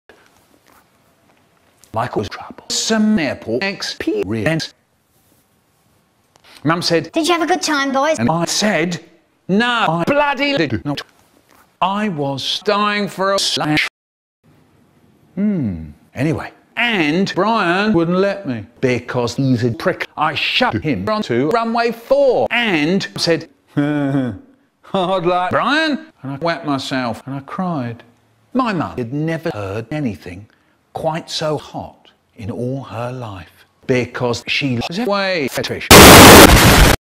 Michael's Troublesome Airport Experience | YTP Quickshot
But I Wanted A Wee.. And They Wouldn't Let Me

Fun Fact: This Video's Script Was Made As A Message To My Friend Adam In A Pure Act Of Bordome. So I Turned It Into An Actual YTP

Unfun Fact: I Spend Hours Just Rendering Each Of The Clips Into Their Own Video To Save Me The Trouble Of Having To Track Down Each Clip In The 6 HD Collections. And Yes, Sometimes I Made Mistakes Or Accidentally Rendered 2 Clips Together So I Had To Re-Render Both Seperately

Here's The Original Script If You're Interested
(Keep In Mind That I Made Some Slight Changes When Putting It All Into The YTP. I Made Some Mistakes In The Original Script Which I Fixed. Also The Lines Aren't Exactly The Same As The Video)
Michael's Troublesome Airport Experience

Mum said "did you have a good time, boys?" and I said "No. I bloody did not. I was dying for a slash-" *Slash appears behind Michael and he turns around*... hmm... anyway "and Brian wouldn't let me because he's a prick so I shoved him onto runway 5 and said "hard luck, Brian"... then I wet myself.. and I cried".. My mum... had never heard anything.. quite so hot.. because she has a wee fetish... *Earraped Logic*